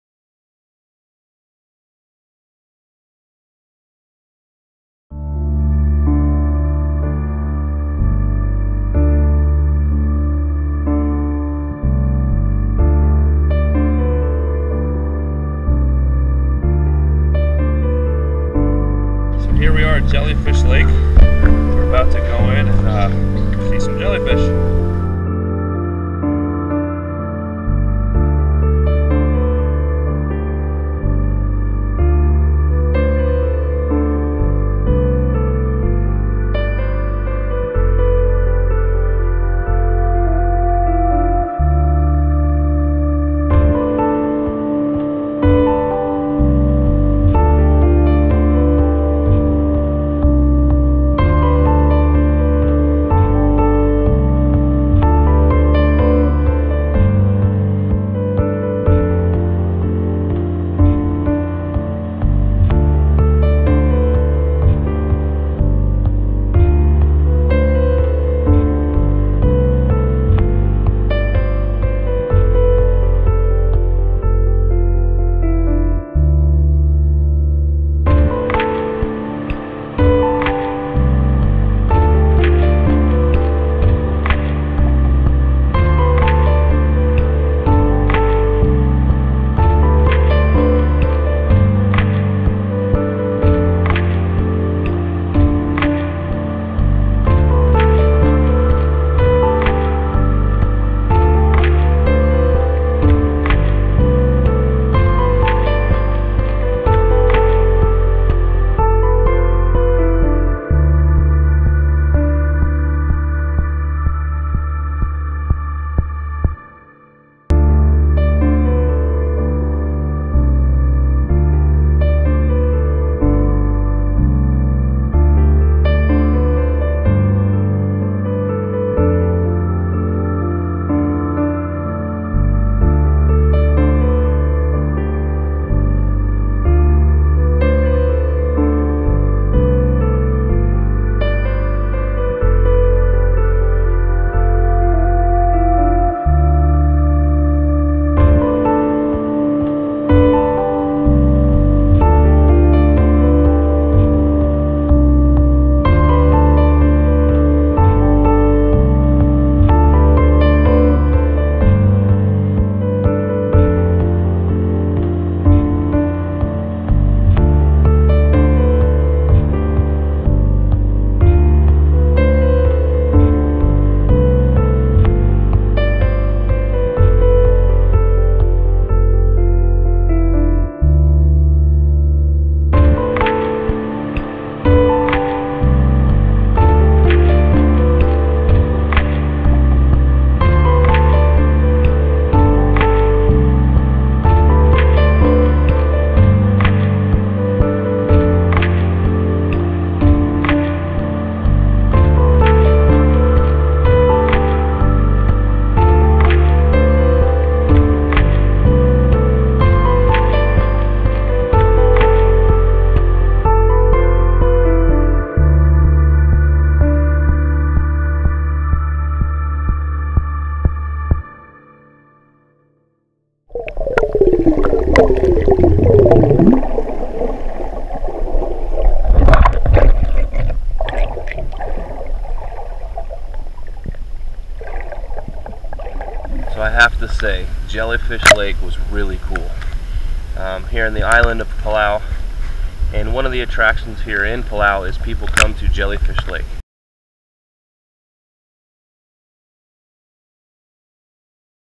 INCREDIBLE !! Underwater Video Jellyfish Lake in the Republic of Palau !!
The AMAZING Jellyfish Lake located in the Republic of Palau in the Micronesia Islands. This video will take you through thousands of Jellyfish and make you feel like you are there swimming through this mystery phenomenon. WATCH closely at the 2:17 mark a jellyfish gets caught up in the air ring and twists to the top. Video Copyright Mike Theiss of